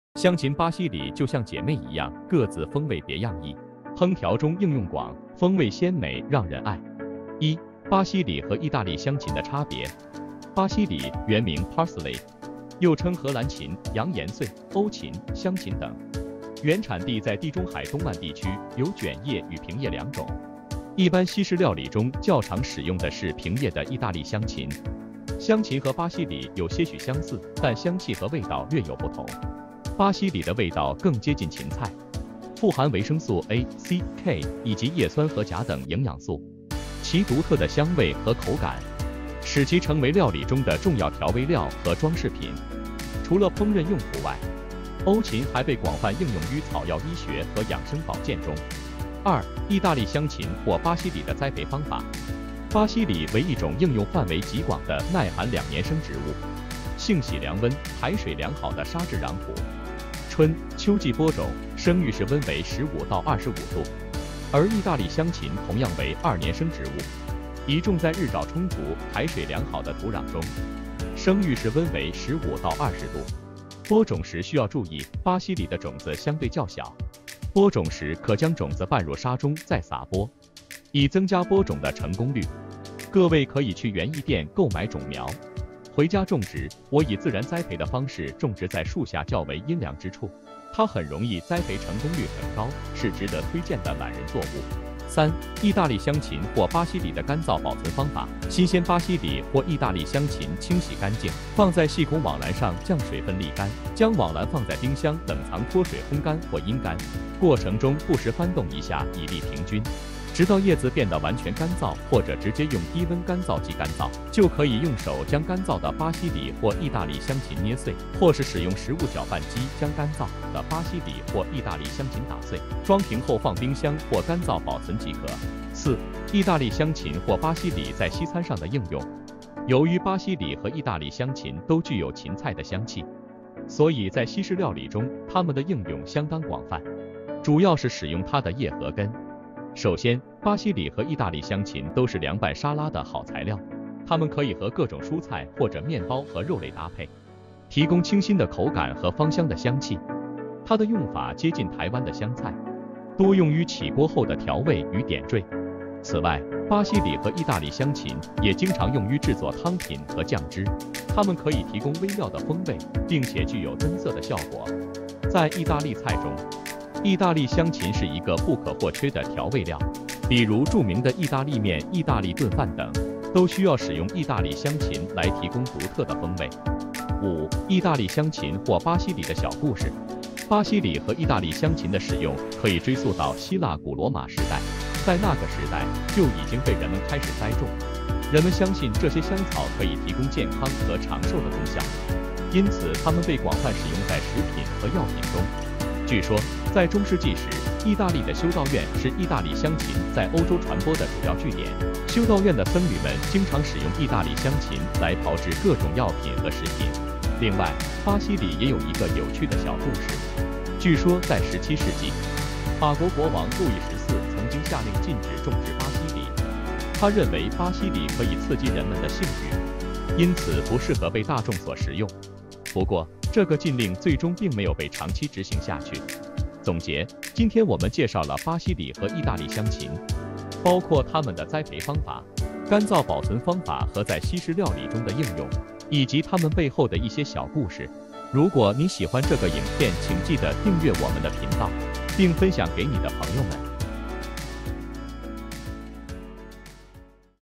巴西里Parsley與義大利香芹栽培與應用事典:你知道巴西里和義大利香芹的差別?今天讓我們來探索完整的認識巴西里與義大利香芹!
香芹巴西里兩姊妹，
各自風味別樣異。
烹調中應用廣，
風味鮮美讓人愛。
1.巴西里和義大利香芹的差別
巴西里，原名Parsley，又稱荷蘭芹、洋芫荽、歐芹、香芹等，原產地在地中海東岸地區，有捲葉與平葉兩種，一般西式料理中較常使用的是平葉的義大利香芹。香芹和巴西里有些許相似，但香氣和味道略有不同，巴西里的味道更接近芹菜。
2.義大利香芹或巴西里的栽培方法
巴西里為一種應用範圍極廣的耐寒兩年生植物，性喜涼溫、排水良好的砂質壤土，春、秋季播種，生育適溫為15～25℃。而義大利香芹同樣為二年生植物，宜種在日照充足、排水良好的土壤中，生育適溫為15～20℃。播種時需要注意，巴西里的種子相對較小，播種時可將種子拌入沙中再撒播，以增加播種的成功率。
3.義大利香芹或巴西里的乾燥保存方法
新鮮巴西里或義大利香芹清洗乾淨，放在細孔網籃上將水份瀝乾。將網籃放在冰箱冷藏脫水烘乾，過程中不時翻動一下以利平均。直到葉子變的完全乾燥或者你可以使用乾燥機直接烘乾，就可以用手將乾燥的巴西里或義大利香芹捏碎，或是使用食物攪拌機將乾燥的巴西里或義大利香芹打碎。裝瓶後放冰箱冷藏或乾燥保存即可。
4.義大利香芹或巴西里在西餐上的應用
由於巴西里和義大利香芹都具有芹菜的香氣，所以在西式料理中，它們的應用相當廣泛。
首先，巴西里和義大利香芹都是涼拌沙拉的好材料。它們可以和各種蔬菜和肉類搭配，提供清新的口感和芳香的香氣。
此外，巴西里和義大利香芹也經常用於製作湯品和醬汁。它們可以提供微妙的風味，並且具有增色的效果。
在意大利菜中，義大利香芹是一個不可或缺的調味料。比如，著名的義大利麵、義大利燉飯等，都需要使用義大利香芹來提供獨特的風味。體驗簡單的生活 農事體驗 農村生活 義大利香芹 巴西里@農夢-竹北小農的成長之路